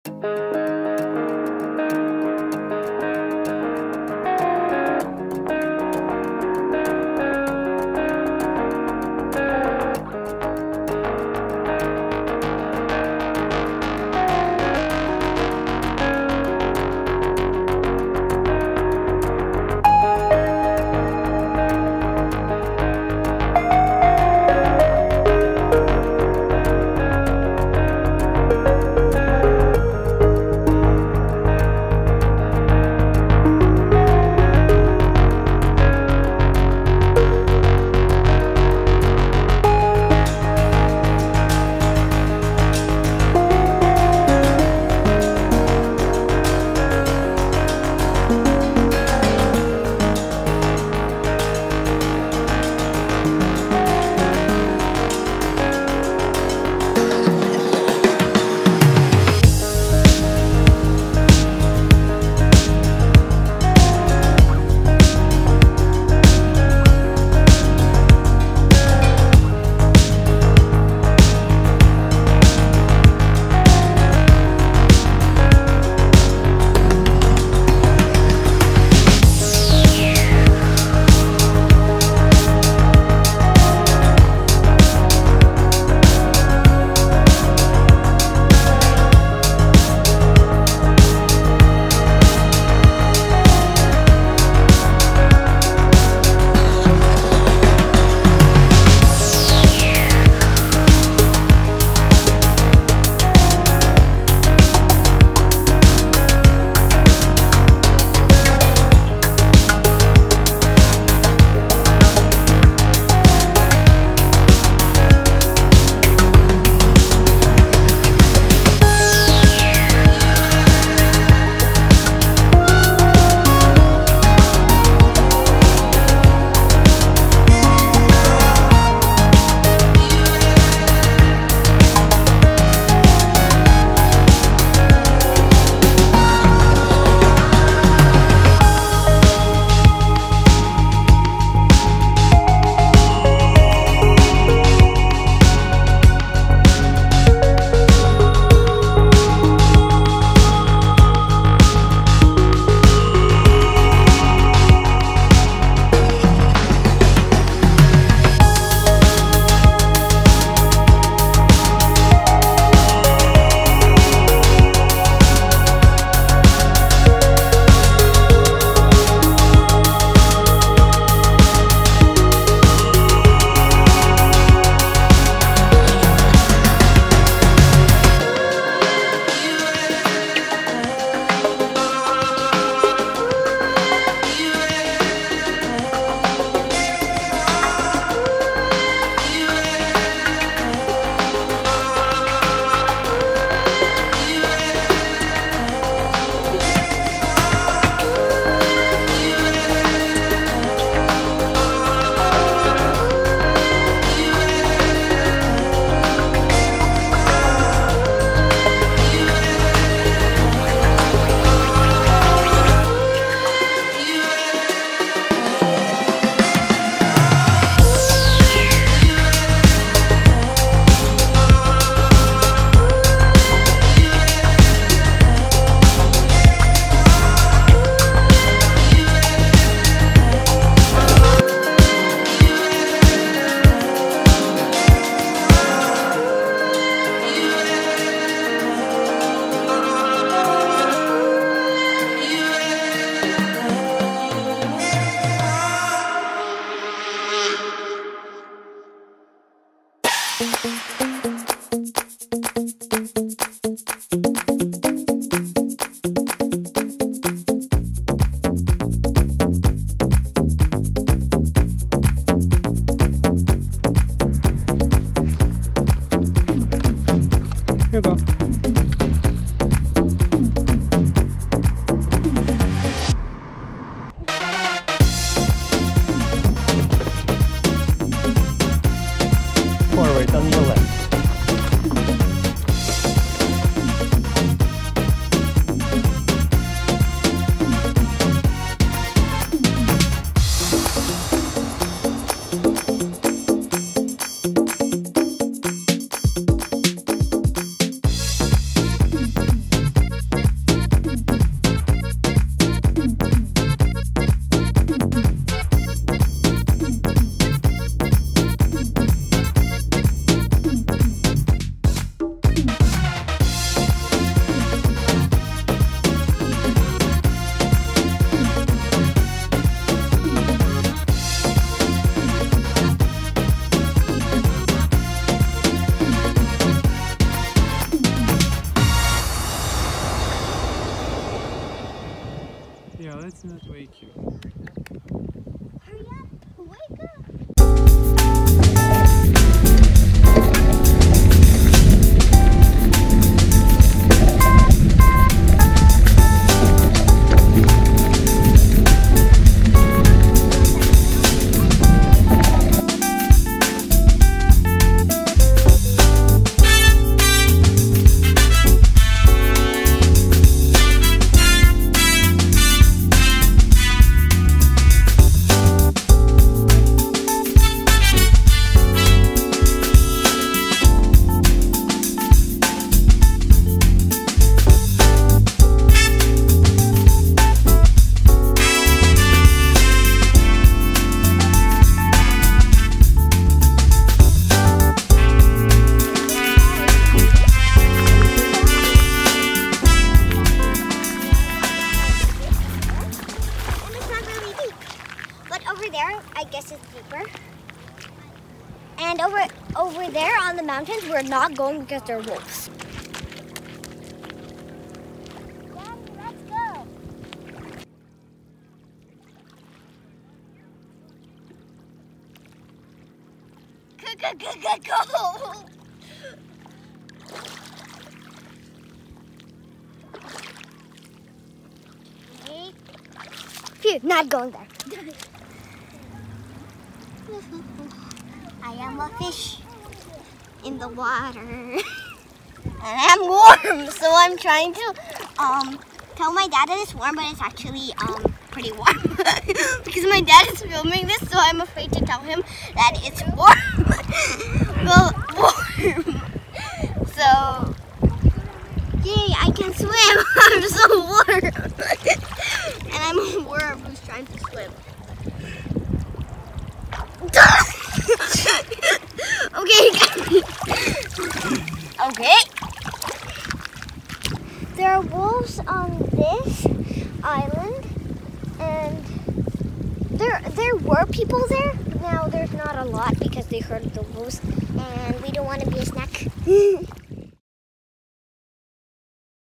Bicycle ride - 06-07-2020
Riding bikes on Lochside trail to Island View Beach. Viewing huge pigs and sleeping otters along the way. My daughter is trying to convince me to swim in the ocean.